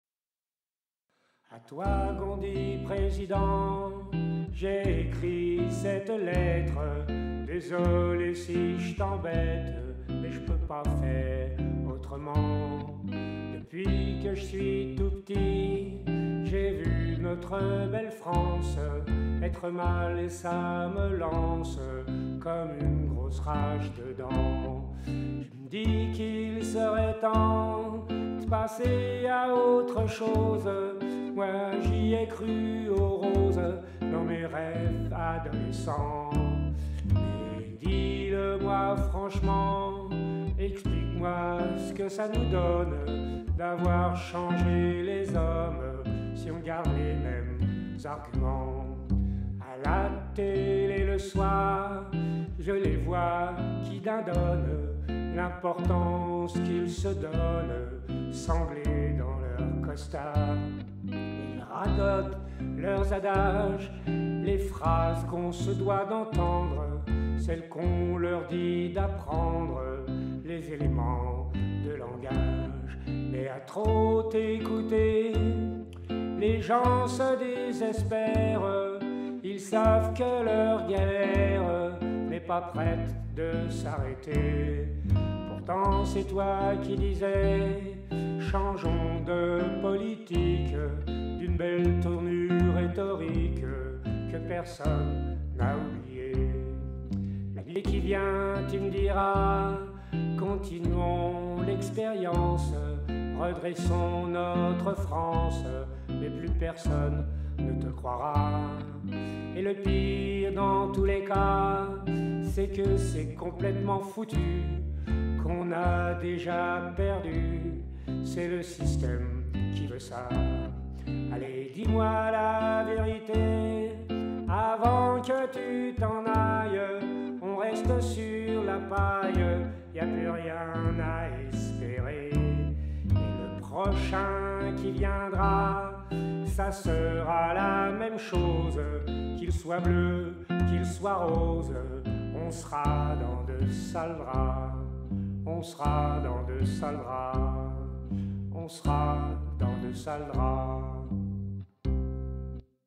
le prez de la rep
Sur une musique de Boris Vian un petit pamphlet politique